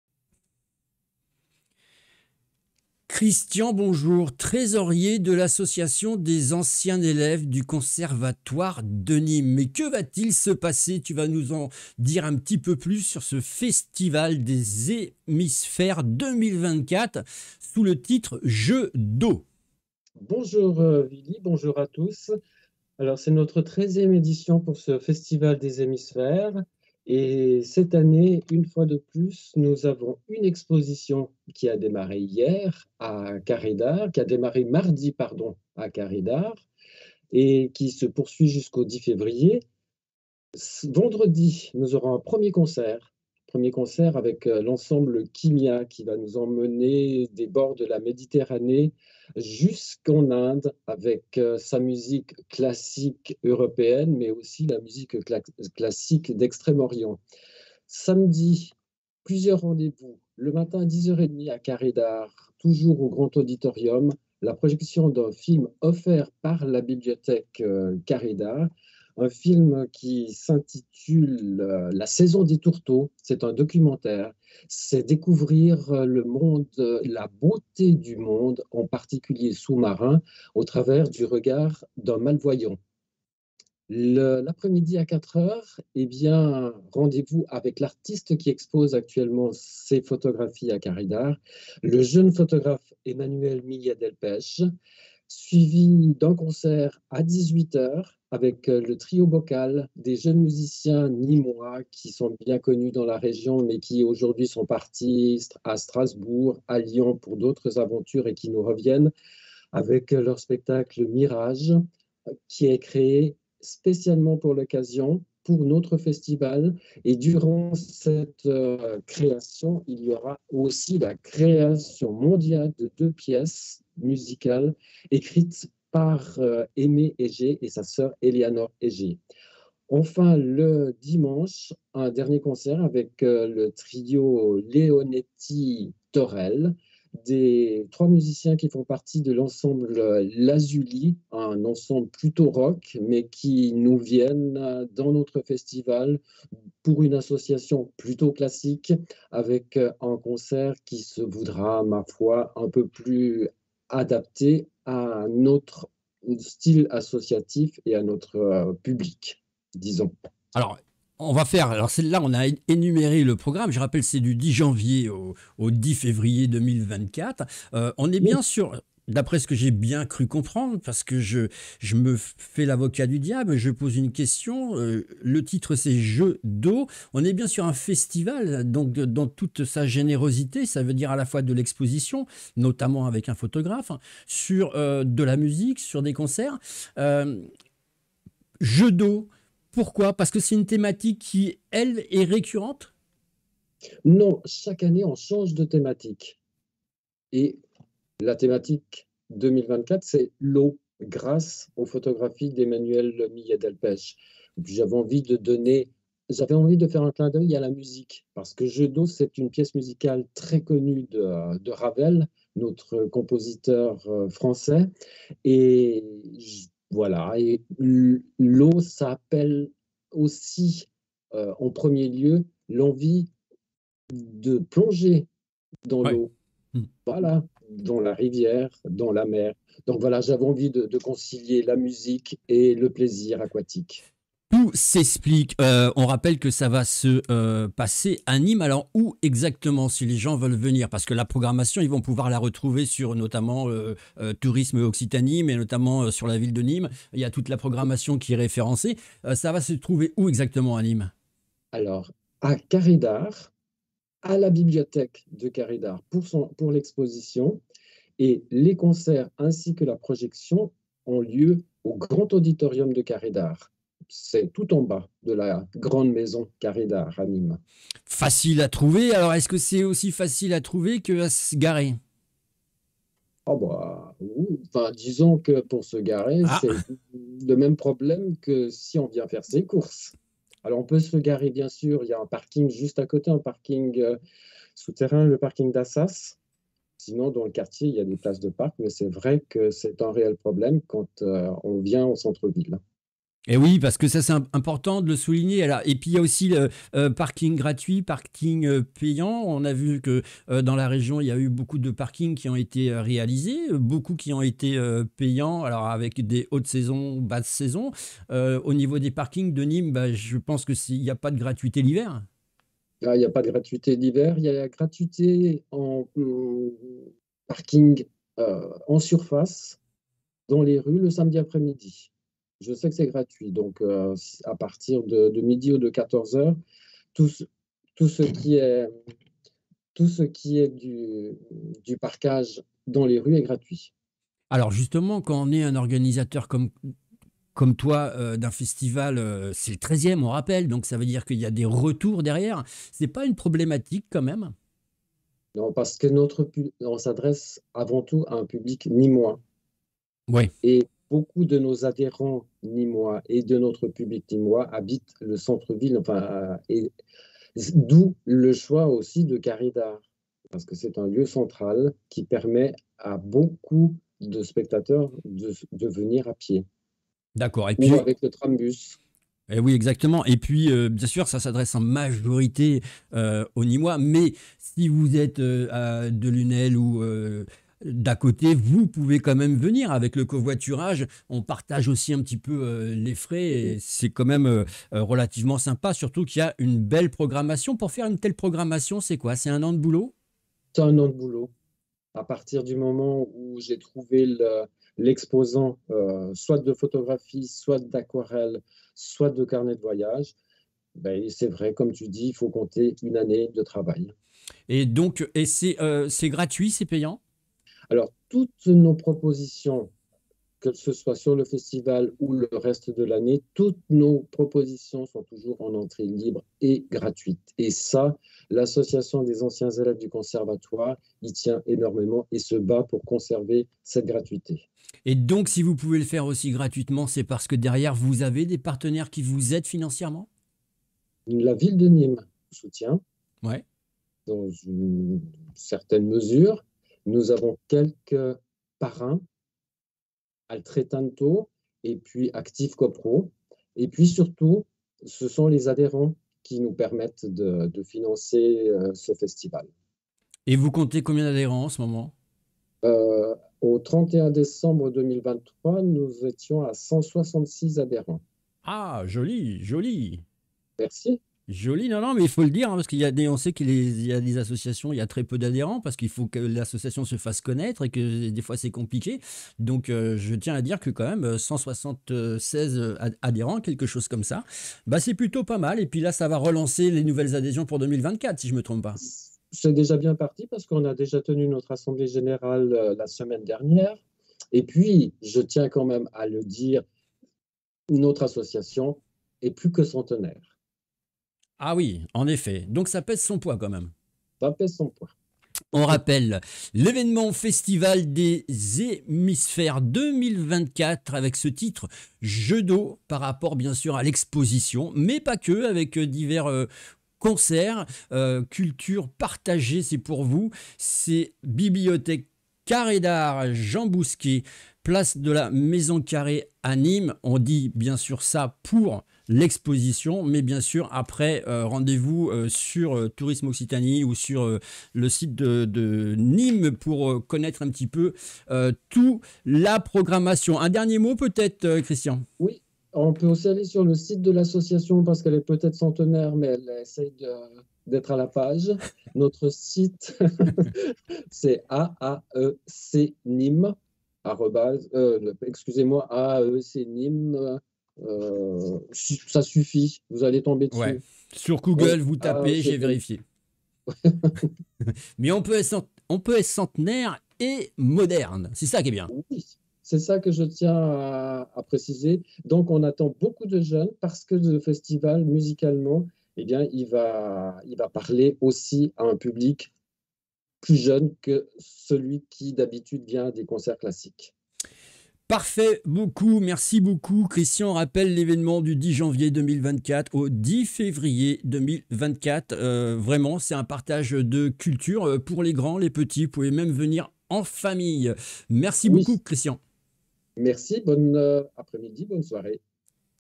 13ème édition du Festival des Hémisphères 2024 - Jeux d'eau à Nîmes du 10/1 au 10/2/24
Interview Christian Corminboeuf, Trésorier de l'association des anciens élèves du conservatoire de Nîmes